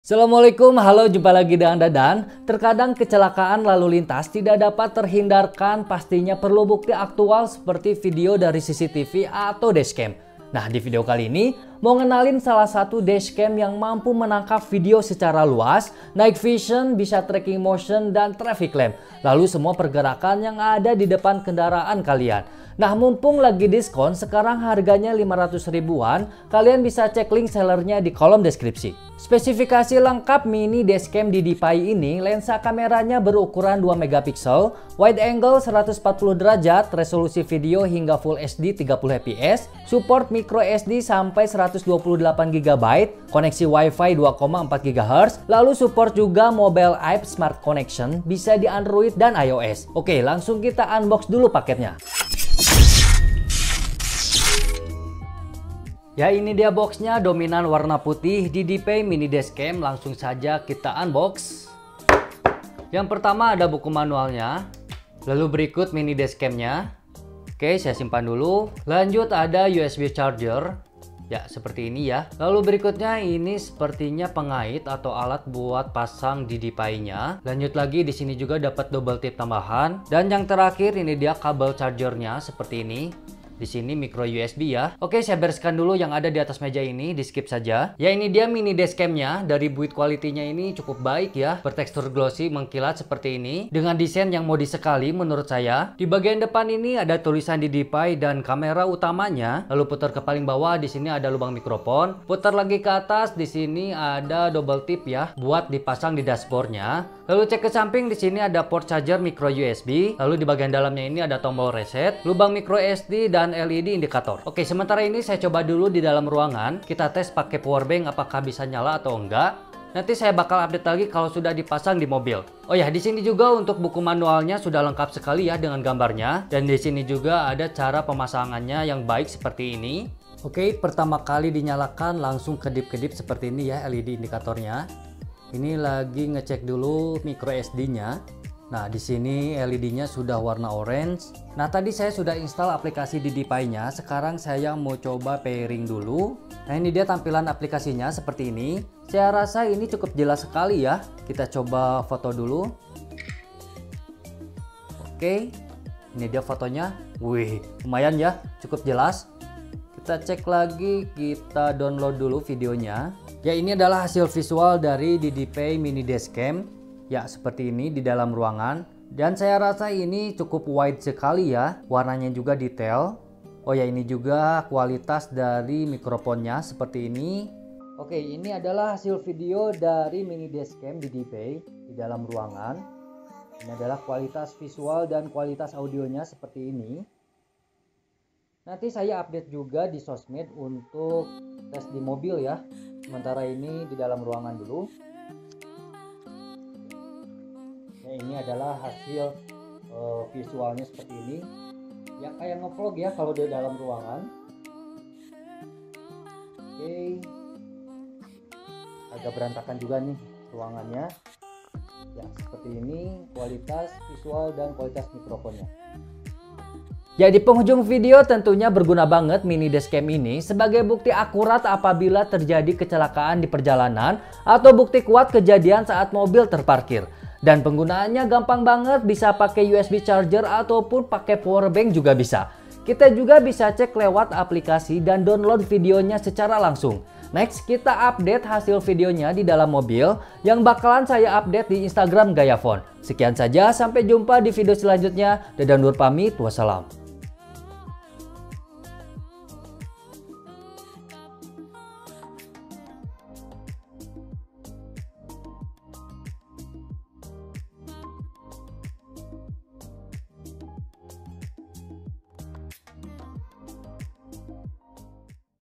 Camera Buat Rekam Kecelakaan Di jalan.. || DDPAI Mini Dash Cam 1080P
alat bukti buat kejadian tak terduga di lalu lintas
produk ini bisa dibeli di :